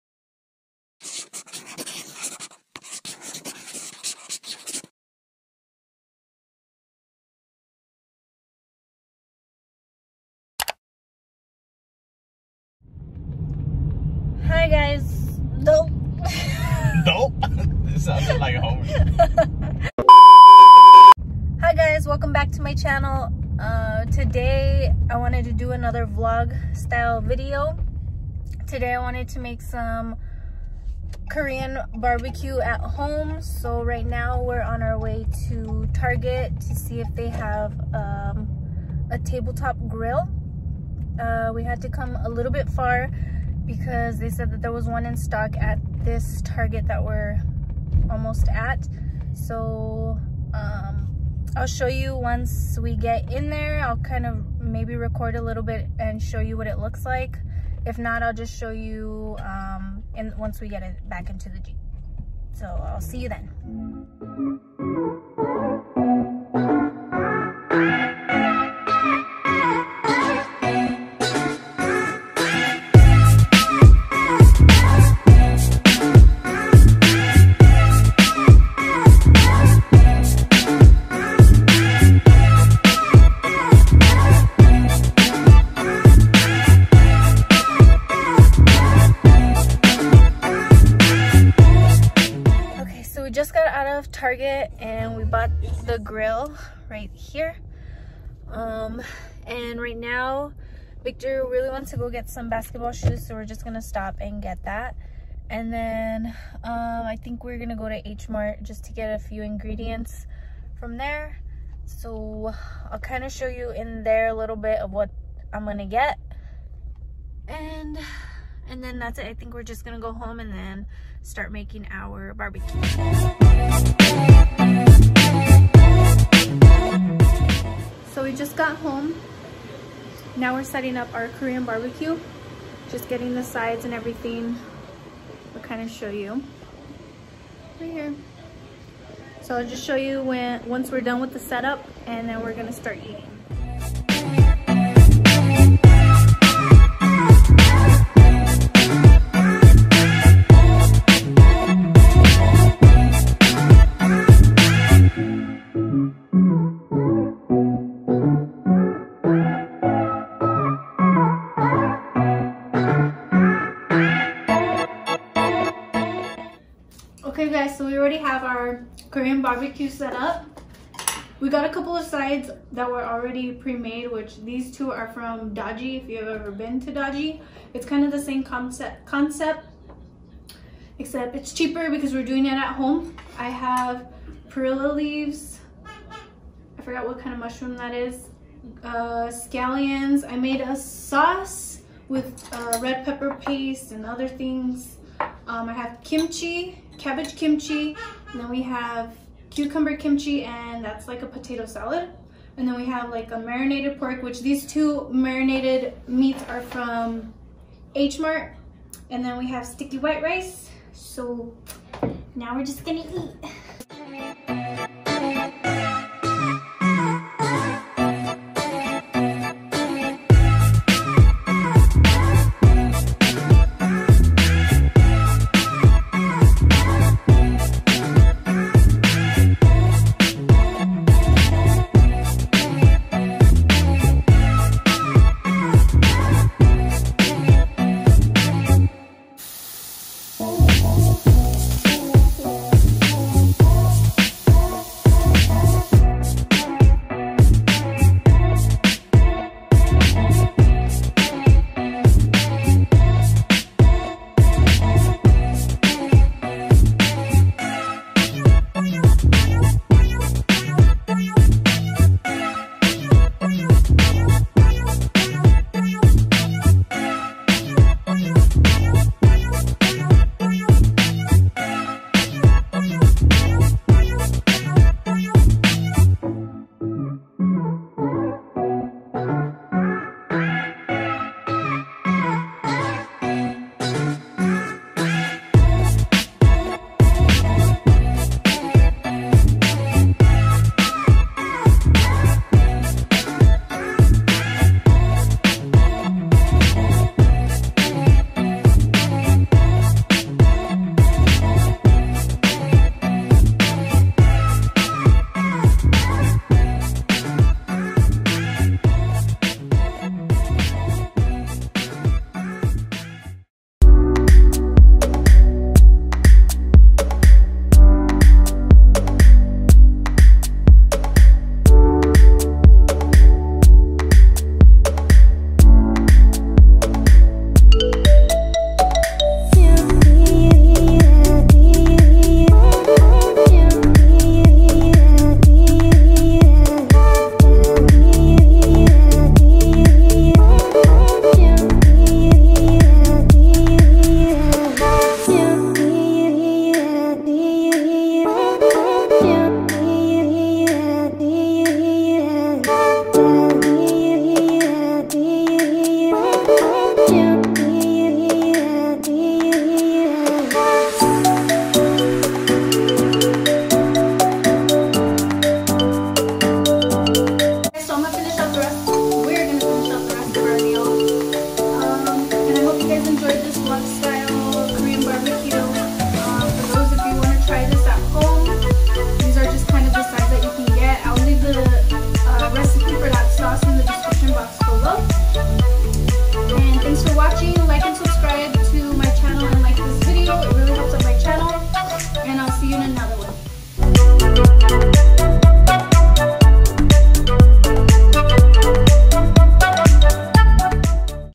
How to make Korean bbq at home
HOW TO MAKE KOREAN BBQ AT HOME/ Here’s is an easy way you can set up Korean style BBQ at home! The smokeless grill shown in this video is from target from the brand chef man.

Spicy Bibimbap Sauce

2 tablespoons gochujang(Korean red pepper paste)
1 tablespoons toasted sesame oil
1 tablespoons brown sugar
1/2 tablespoon soy sauce
1/2 tablespoons water
1 teaspoon rice vinegar
1 garlic clove minced
1/2 tablespoon toasted sesame seeds(optional)